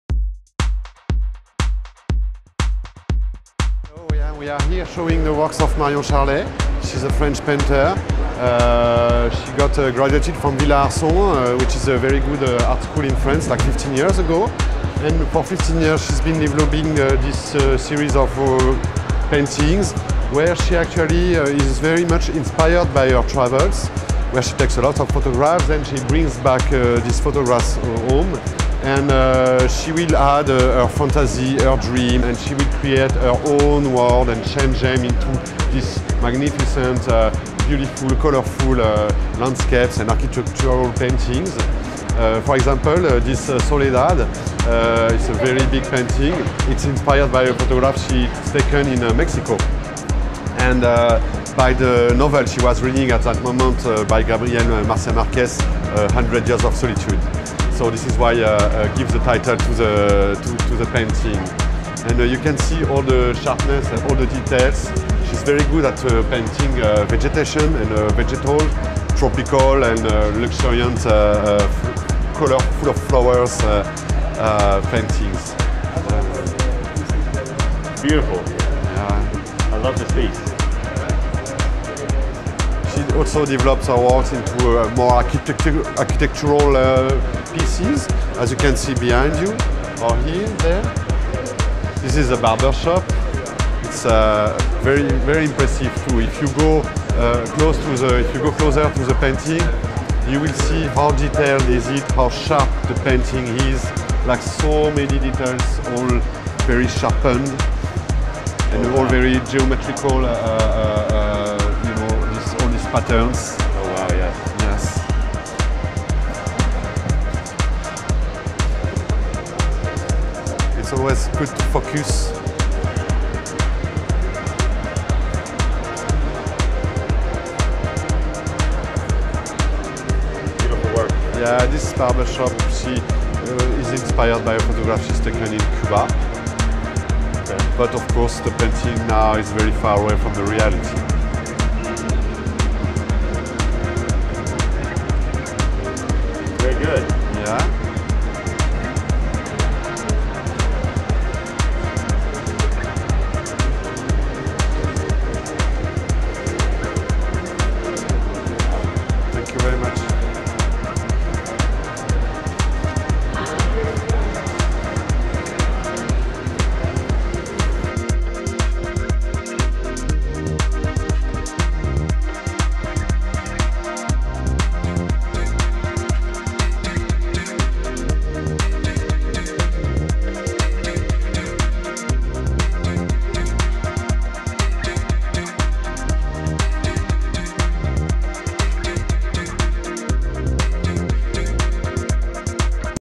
Miami Art Week 2021 | Untitled Art Fair | PARIS-B | Marion Charlet | Architectural paintings
00:00 - intro
00:06 - Marion Charlet France
00:17
00:28 Villa Arson graduate
01:00 - Architectural, tropical, flora paintings
02:30
02:47